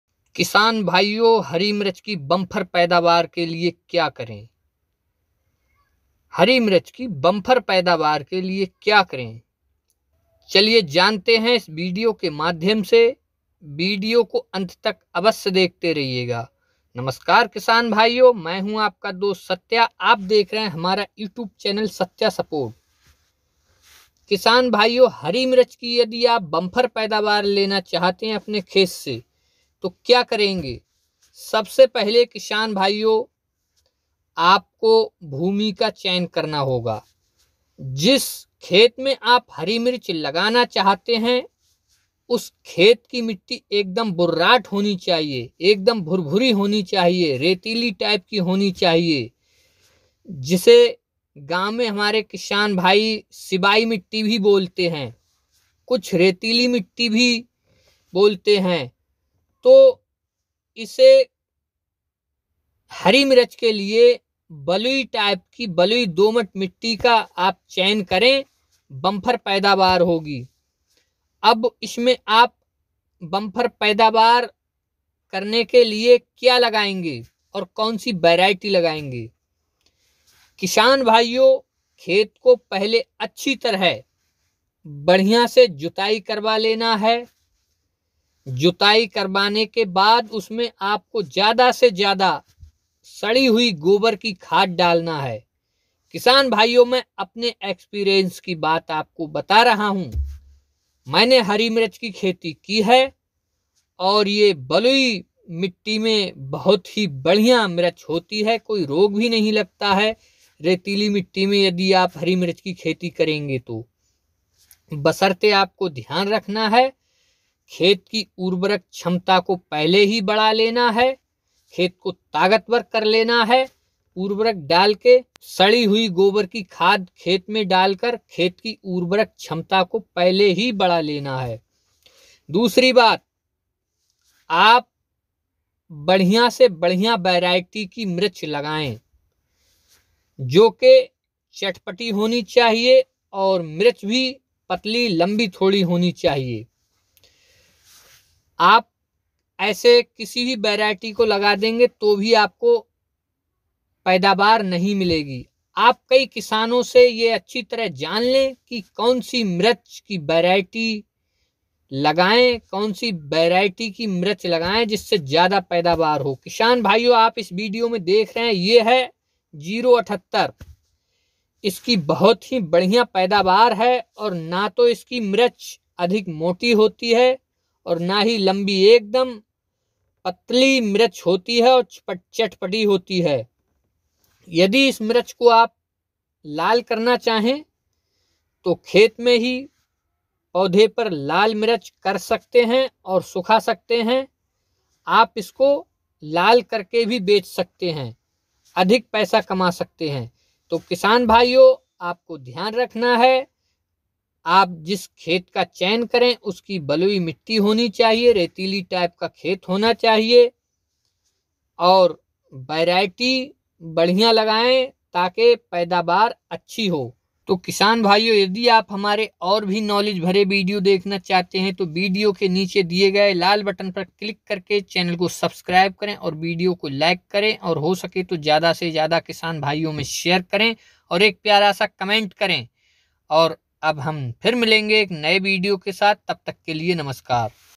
hari mirch ki kheti ke baare mein jankari
Hari mirch ki bumper paidawar ke liye Kya Karen
namaskar Kisan bhaiyon
avashyak Suchna.
Kisan bhaiyon is video mein Humne bataya hai ki hari mirch ki bumper paidawar Kaise len is video mein Humne share Kiya Hai Apna practical knowledge Jo par Humne zero mirch Lagaye hai aur uski bumper paidawar Humko Mili Hai hari mirch Lagane ke liye Kis Tarah Ki Bhumi honi chahie Kis Tarah Ki Bhumi Mein paudhe Lagane chahie aur kaun kaun si variety Lagani chahie is video mein Humne Aapke bich share Ki Hai yadi kis Jankari se aap Kisi Kisan bhai ko koi Labh ya Hani hota hai to Uske jimmedar Ham Nahin Honge aur na hi Hamara channel Hoga video dekhne ke liye dhanyvad